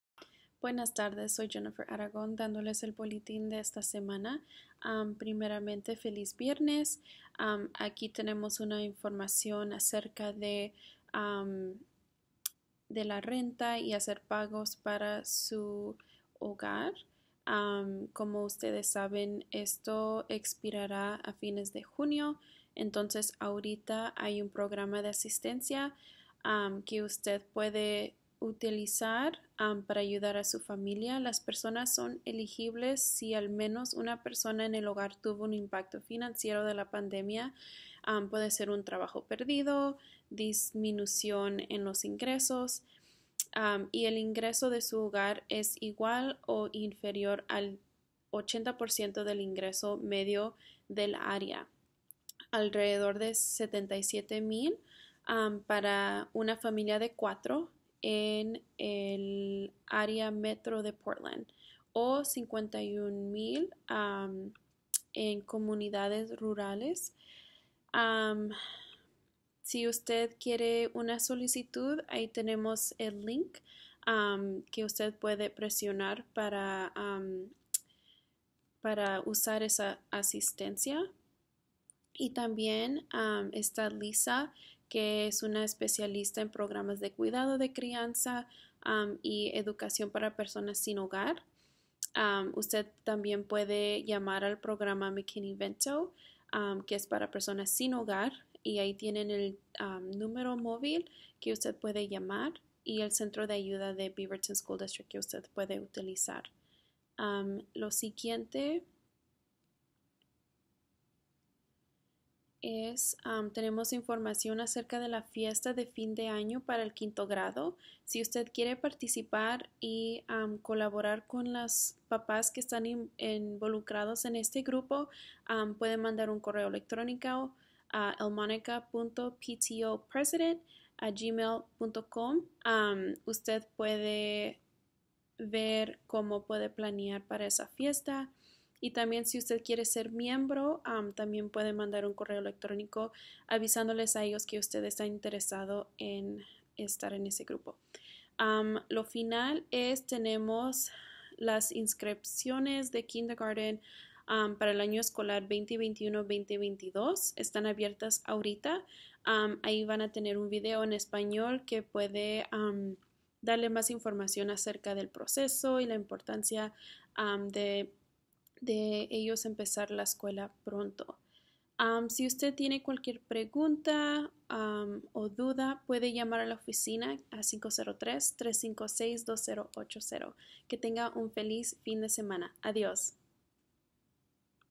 Boletín Elmonica - 21 de mayo de 2021
Mayo es el Mes de la Herencia Judía Estadounidense
Información de ayuda para inquilinos
Calendario por mes
Calendario del año escolar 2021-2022
Encuesta sobre rutas seguras a la escuela
Recursos